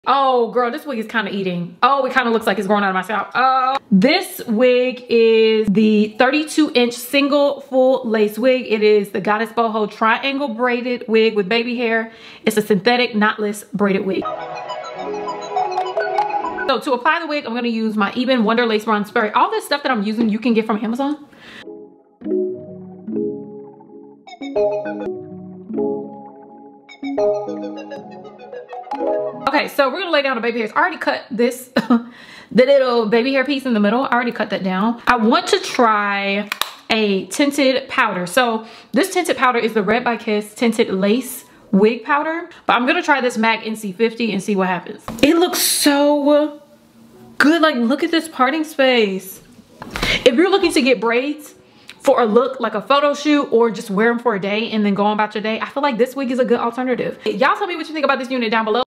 AMAZON BRAIDED WIG FIND?!
WIG #1 PIB BOHO BRAIDED WIG WITHN TRIANGLE PARTS
color: 2
Length: 32 inches
price: $149.99